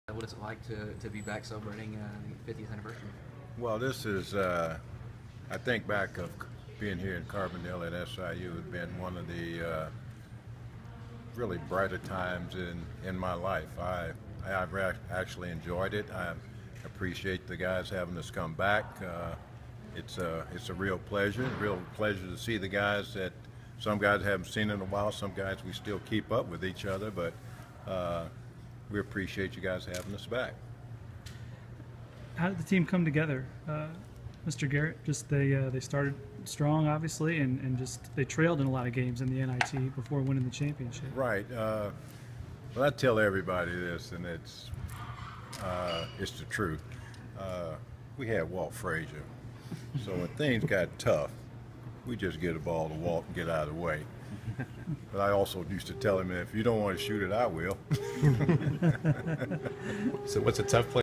Dick Garrett remembers SIU's 1967 NIT championship
Former Southern Illinois University guard Dick Garrett, who played five years in the NBA, talks to the media Feb. 10, 2017, before a banquet honoring the Salukis' 1967 NIT championship team.